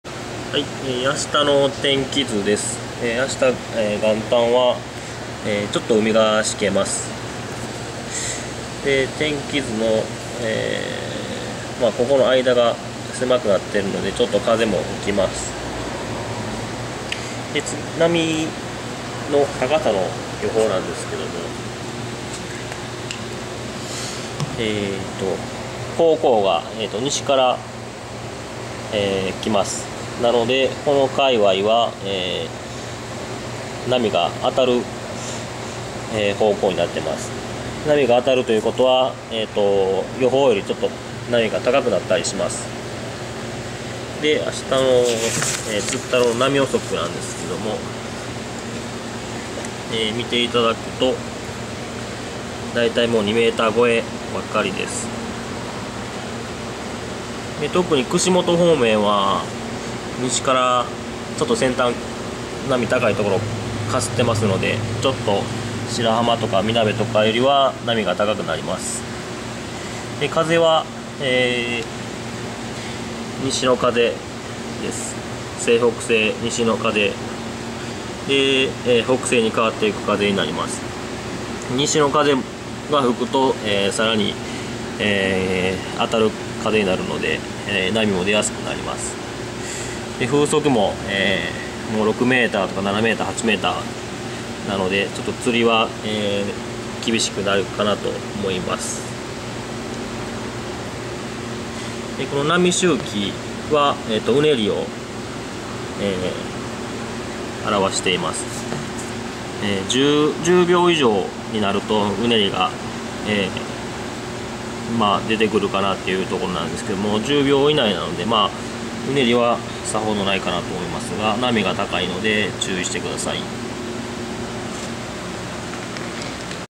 ２０１４年元旦の波・風の解説
元旦の風と波です。次第に冬型の気圧配置になり、西から北西の風に変わります。